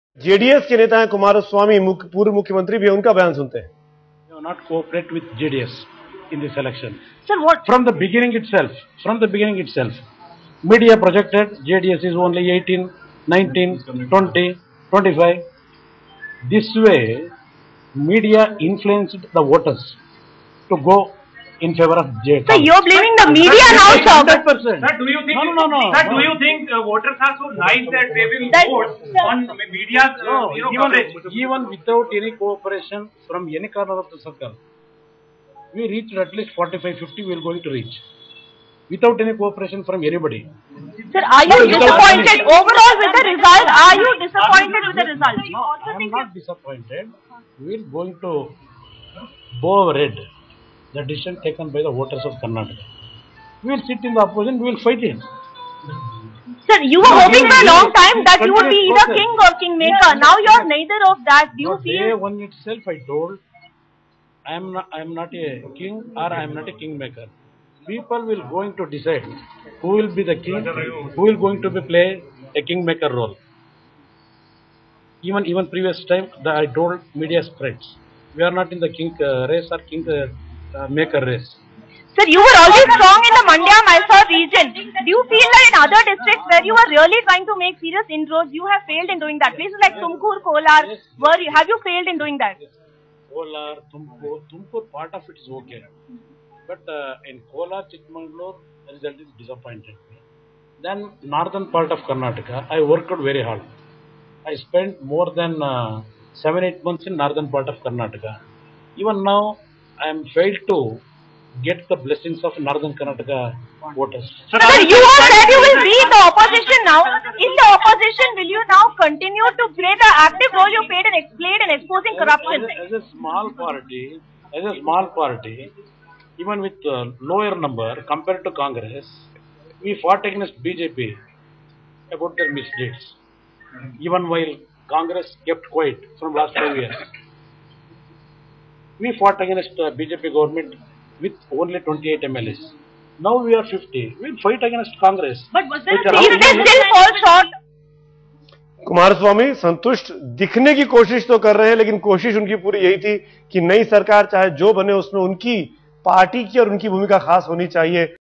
JDS chief Kumaraswamy blames media for defeat in Karnataka
JDS chief Kumaraswamy has blamed media for his part defeat in Karnataka Assembly polls.